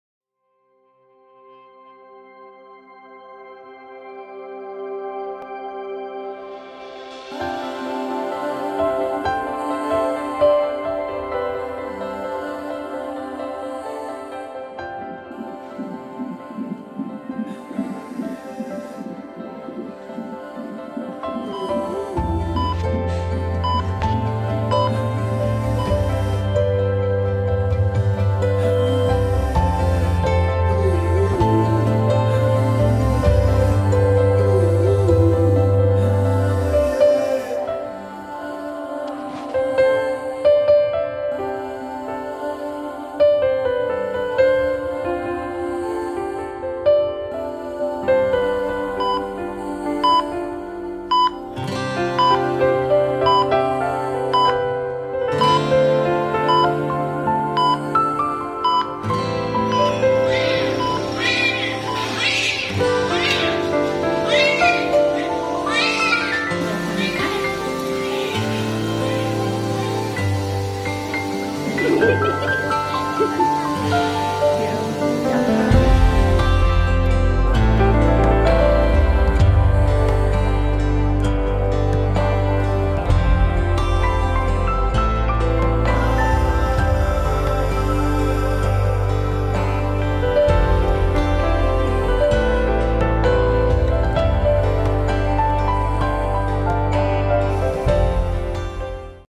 Kanaka Wisesa Story | Vivo V19 Video Test
Vivo V19 Video Test record di RS saat detik-detik kelahiran anak ke 2
kamera Vivo v19 lumayan baik dari segi warna dan stabilizre nya cukup membantu.